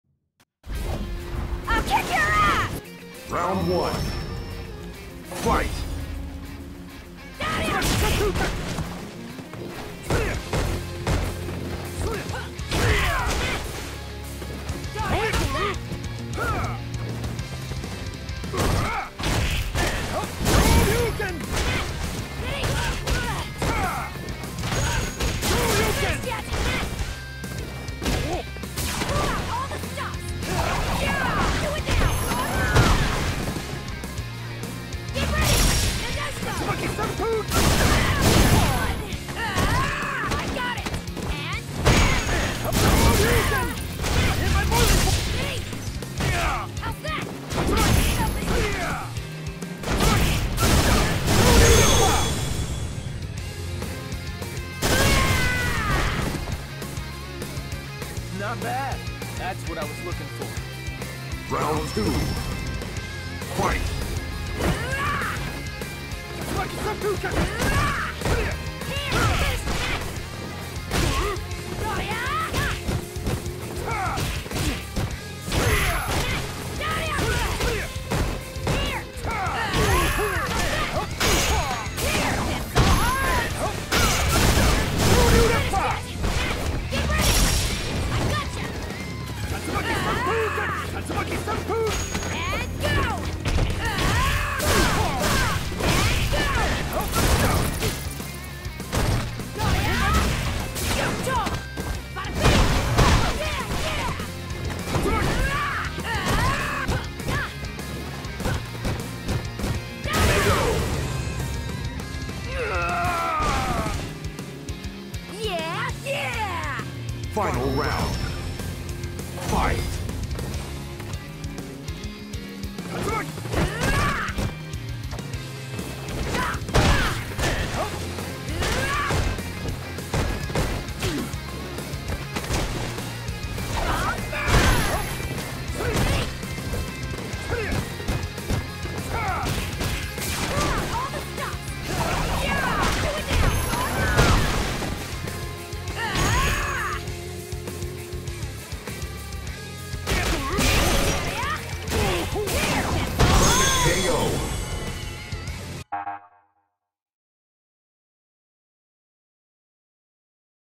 Even Friskier Extra (/°¤º)/~Rage quitter "Insight403"~\(º¤°\)
The Ranked grind is hard enough without Rage quitters like Insight403 stealing points & getting win streaks like a scrub but do you want to know the sad truth is? I was feeling down after losing a ton of points practicing with several characters on ranked until the scrub Insight403 appeared with that ugly Ken outfit on. Thanks Insight403 casually destroying you without any efforts made me feel Even Friskier!